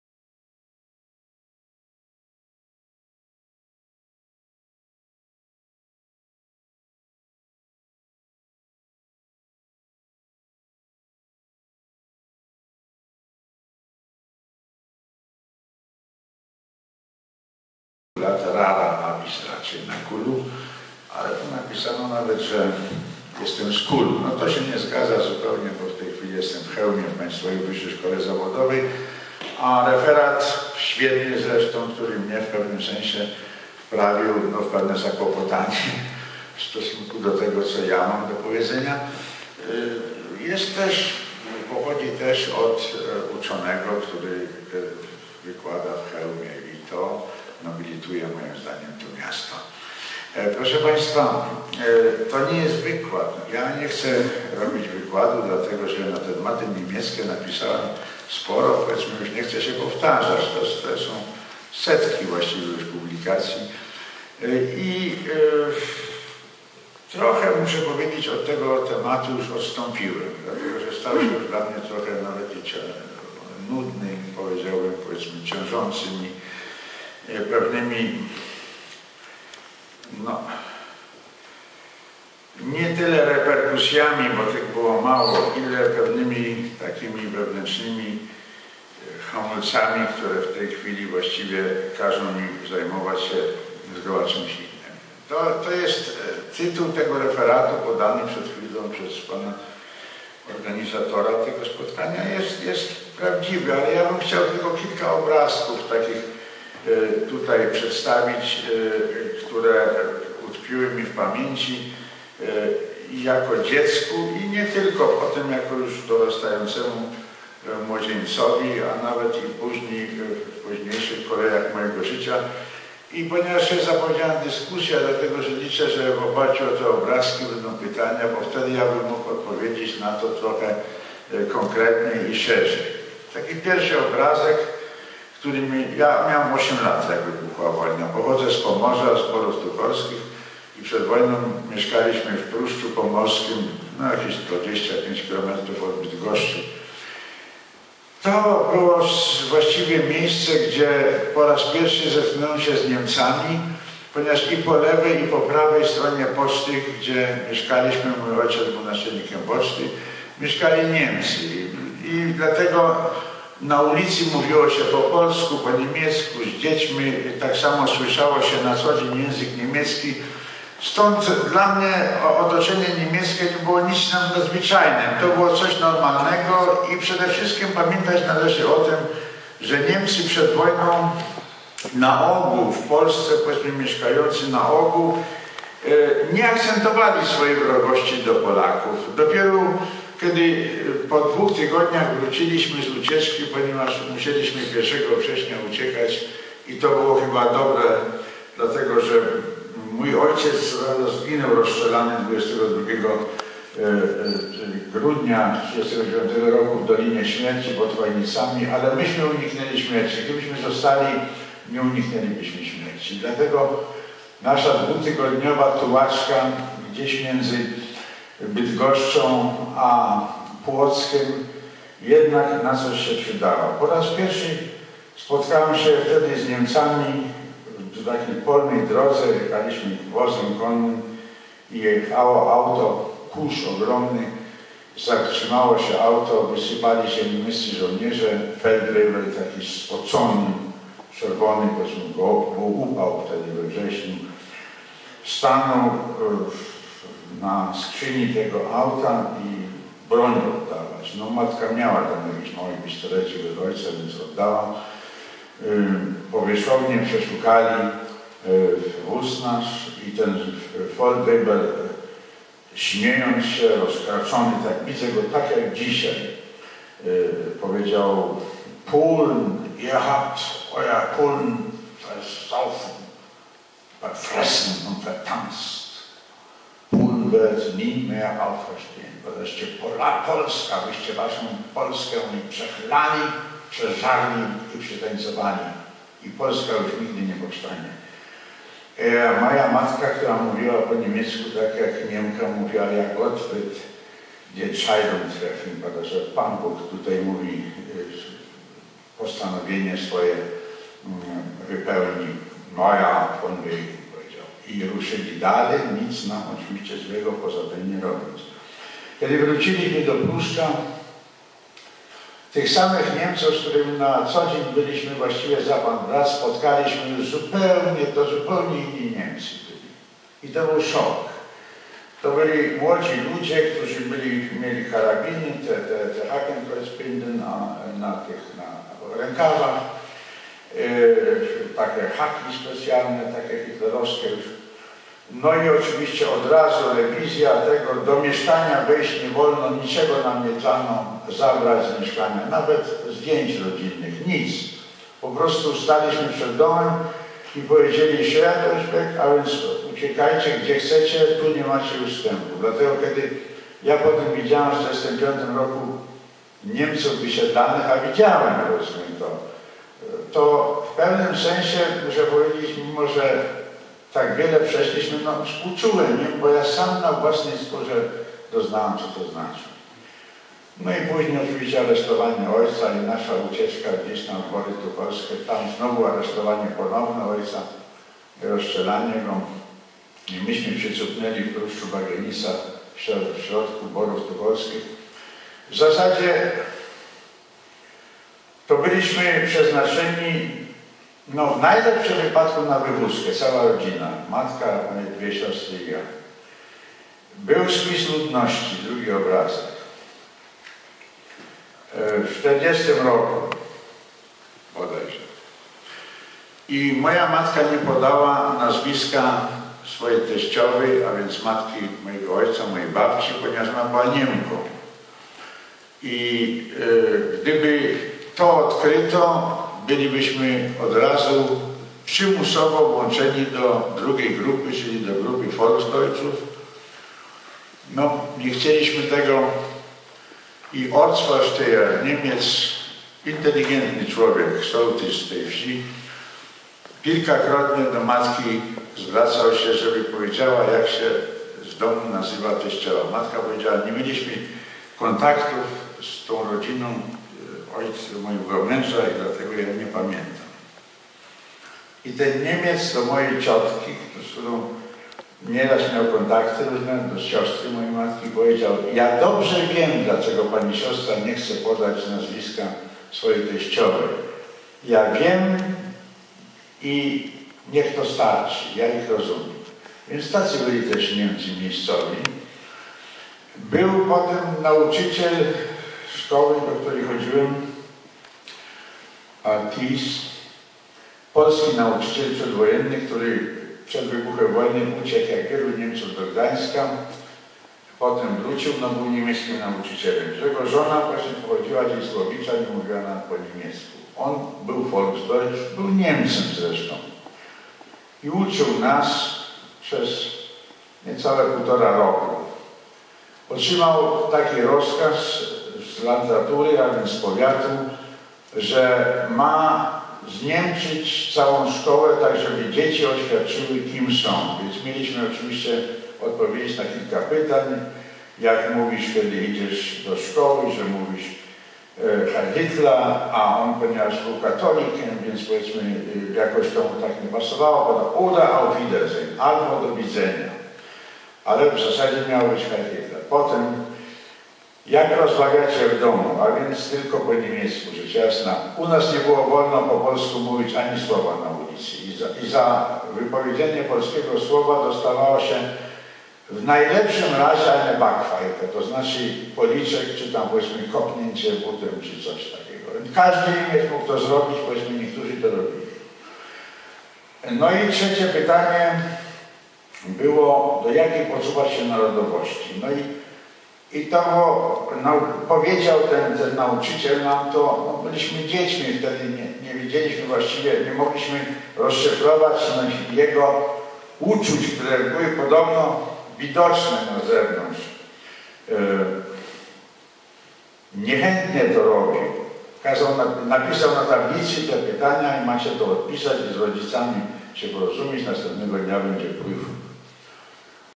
"Okupacja niemiecka oczami dziecka szkolnego" 1/3
Wystąpienie ks. prof. dr hab. Zygmunta Zielińskiego zatytułowane: "Okupacja niemiecka oczami dziecka szkolnego" wygłoszone 13 listopada 2009r. w Katolickim Uniwersytecie Lubelskim Jana Pawła II w czasie konferencji naukowej "Doby - zły. Relacje polsko-niemieckie 70 lat od wybuchu wojny."